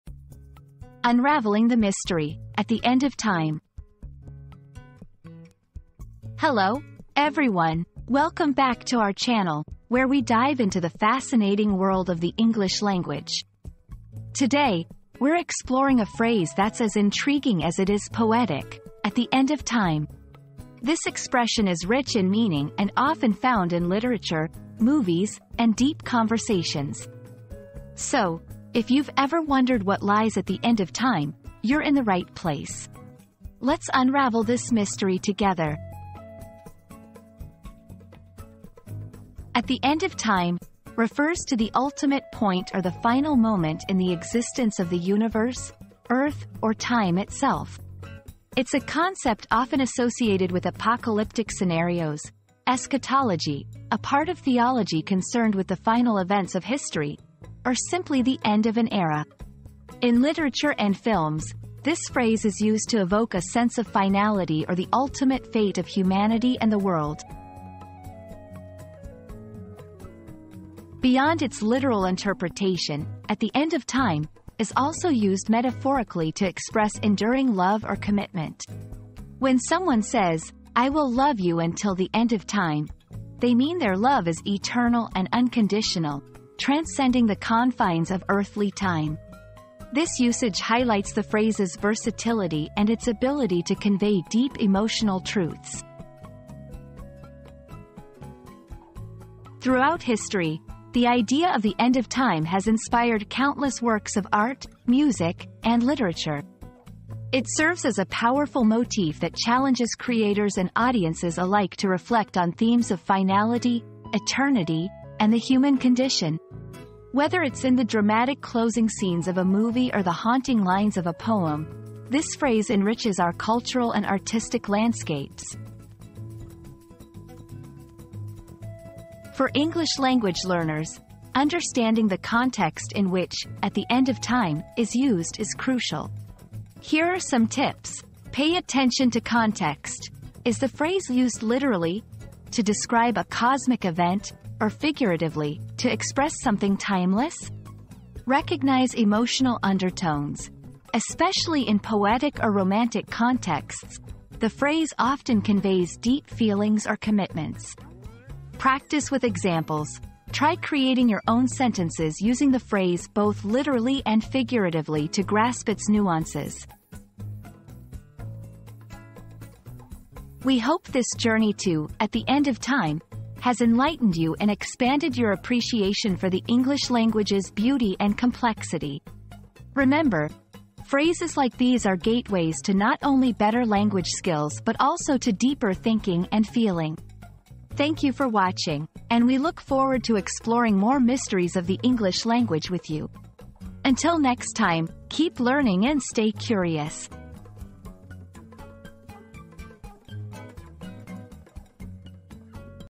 Unraveling the Mystery: "At the End of Time"
00:00 • Introduction - Unraveling the Mystery: "At the End of Time"
00:39 • The Literal Meaning
01:10 • The Figurative Use
01:39 • Cultural and Artistic References
02:08 • Usage Tips for English Learners